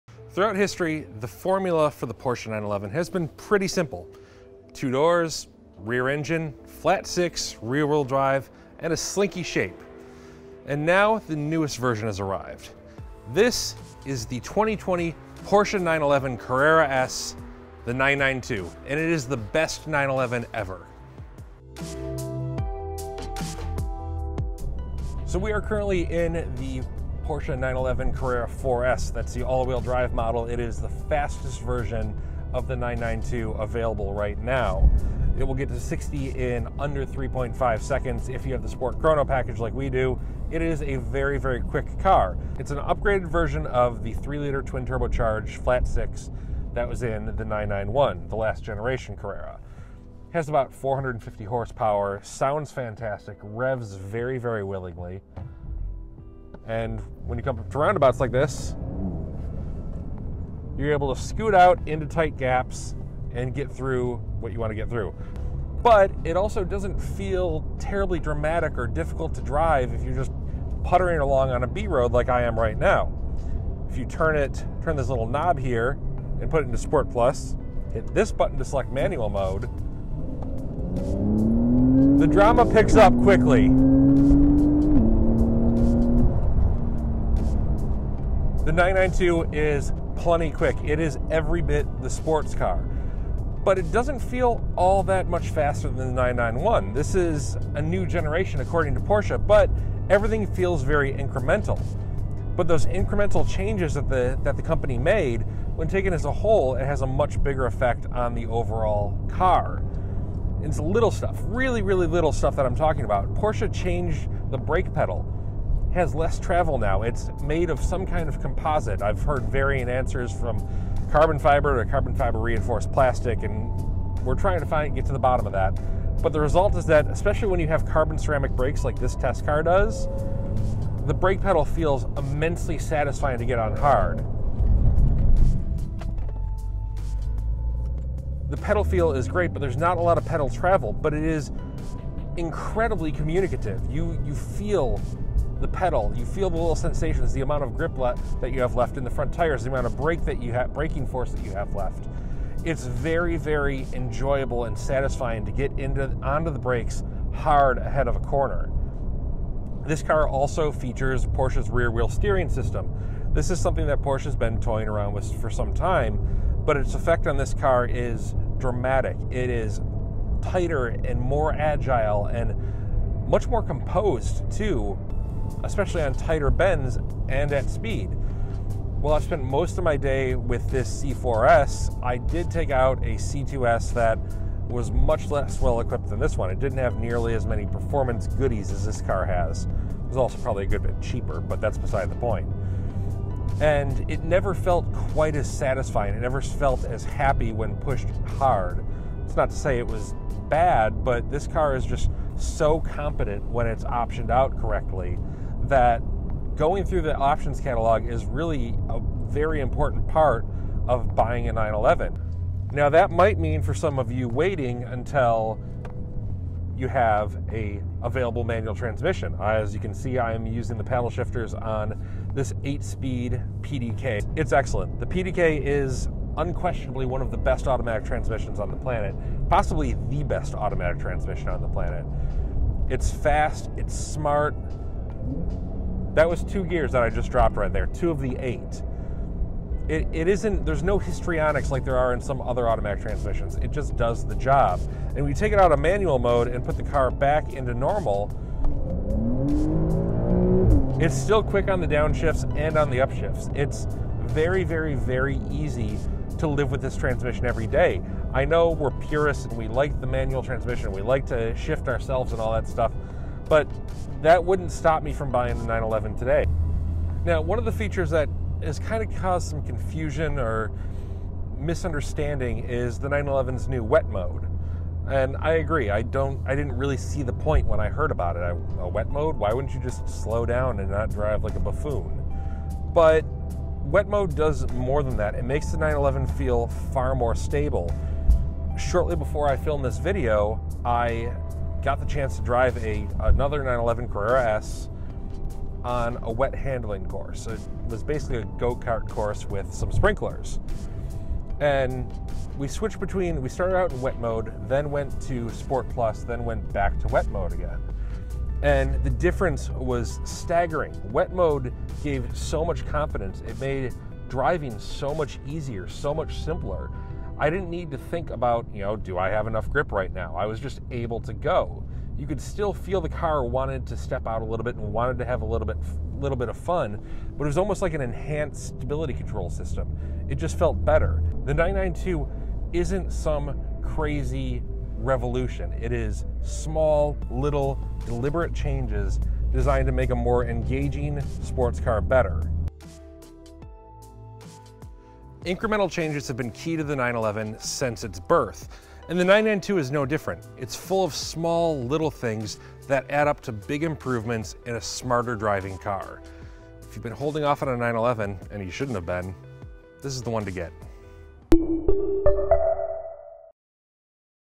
2020 Porsche 911 (992) Carrera 4S First Drive
Motor1.com managing editor, Brandon Turkus drives the all-new Porsche 911 (992) in Spain.